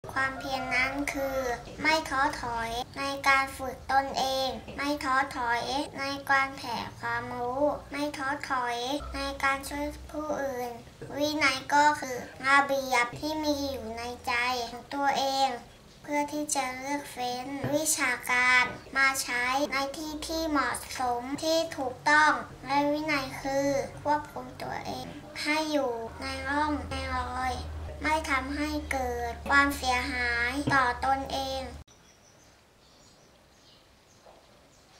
คำพ่อสอน:"ความเพียร" ผู้อัญเชิญ [ปานวาด เกษมสุข] ๙ตามรอยพ่อ
words-from-king
คำพ่อสอน พระบรมราโชวาท /พระราชดำรัส ในพระบาทสมเด็จพระเจ้าอยู่หัว รัชกาลที่ 9
ที่ทรงพระราชทานแก่ พสกนิกรชาวไทย ในหลายๆโอกาส
น้อมนำพระบรมราโชวาทเป็นแนวทางในการดำรงชีวิต :

พระบรมราโชวาท
พระราชทานแก่คณะครูฝึกตำรวจตระเวณชายแดน
ณ ศาลาเริง พระราชวังไกลกังวล
วันพุธที่ ๒๑ พฤษภาคม ๒๕๑๘

อัญเชิญโดย ด.ญ.ปานวาด เกษมสุข
นักเรียนชั้นอนุบาล 2 โรงเรียนบ้านดอนด่านใน
ต.ด่านใน อ.ด่านขุนทด จ.นครราชสีมา

Royal Guidance
The Royal Guidance of His Majesty the King Bhumibol Adulyadej